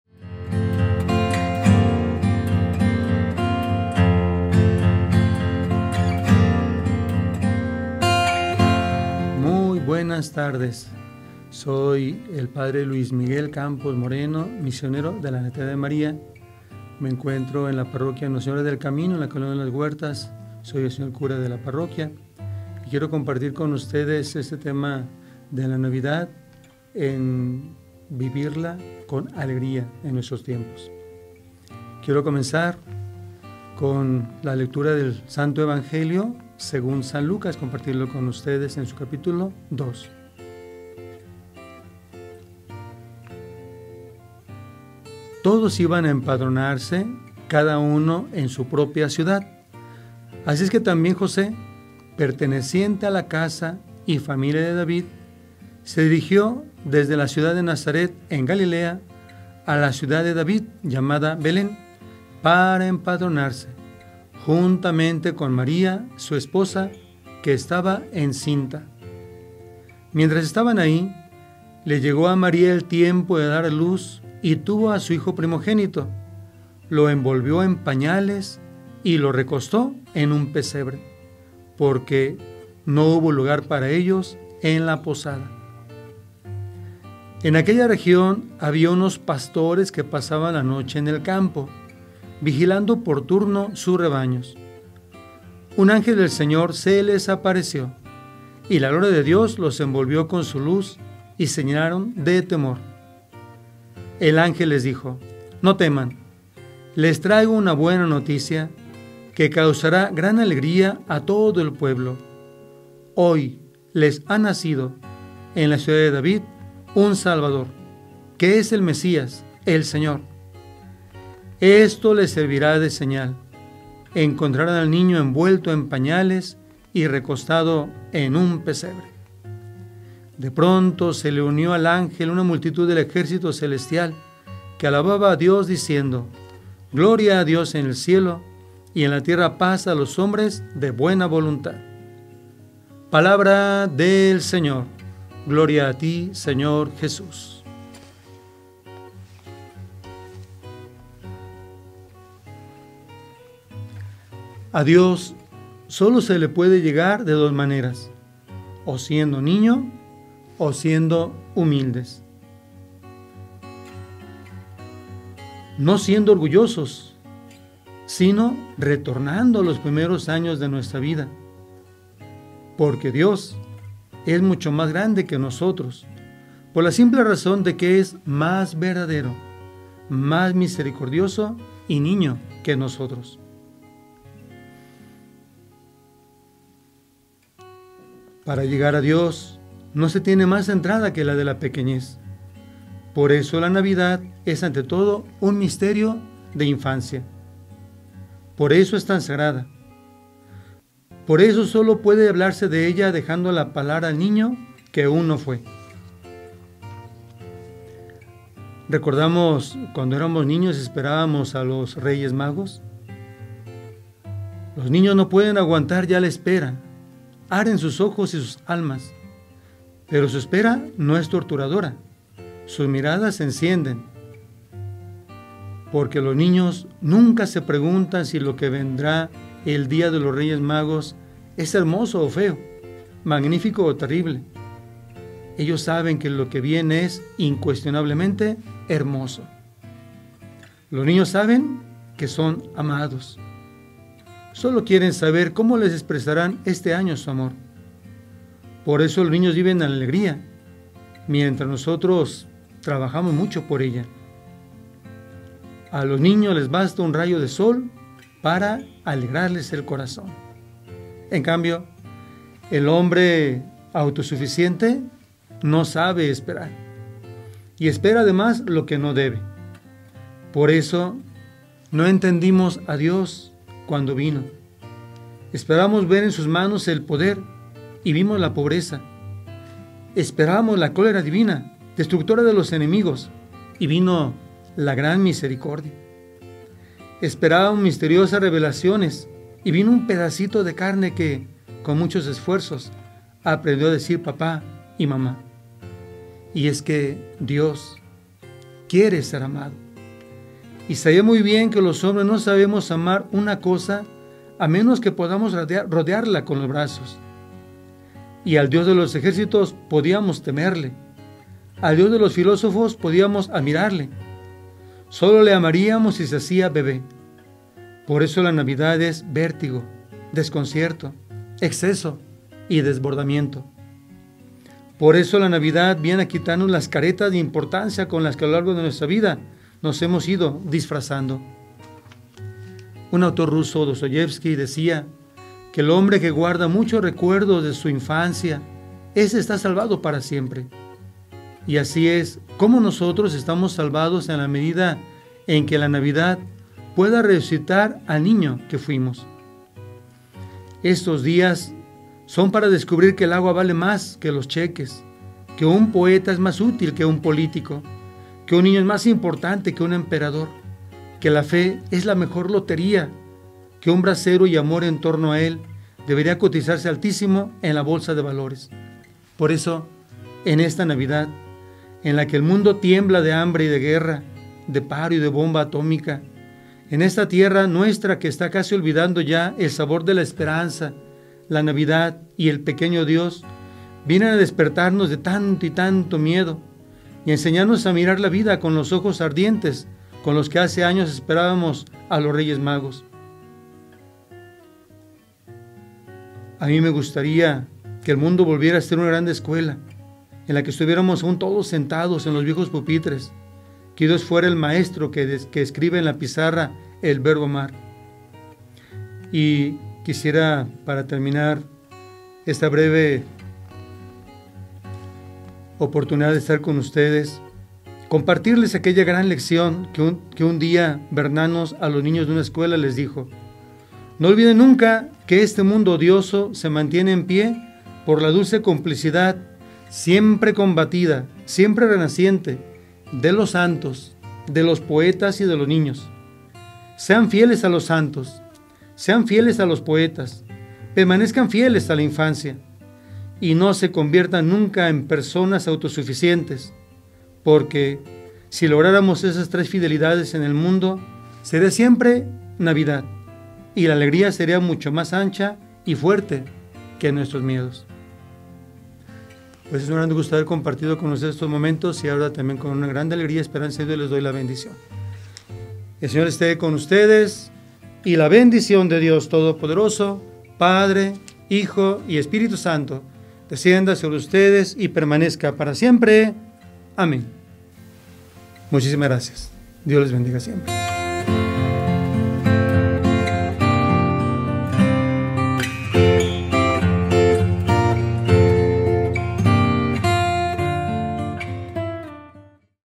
Vivir la Navidad con alegría en nuestros tiempos
El P. Luis Miguel Campos Moreno, Misionero de la Natividad de María (M.N.M.) nos invita a reflexionar sobre el Evangelio de Lucas 2, 10-12, Vivir la Navidad con alegría en nuestros tiempos.